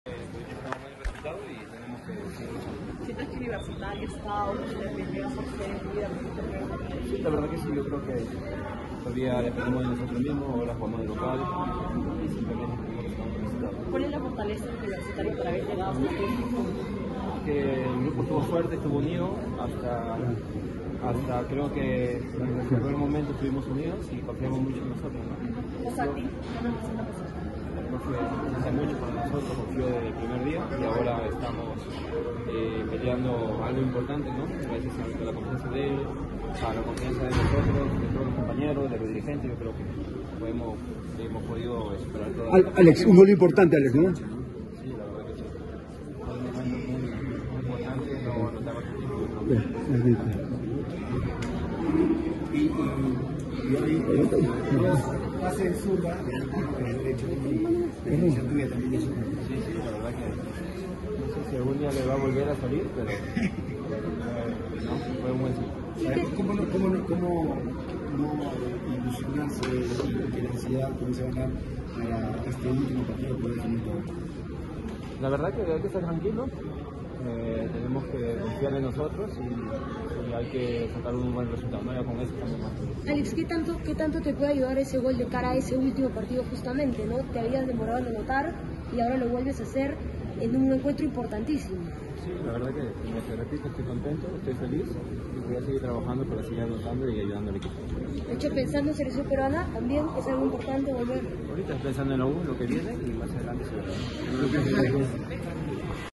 Alex Valera: "Hay que estar tranquilos, dependemos de nosotros"
El delantero merengue marcó el empate sobre el final del partido ante Cusco FC.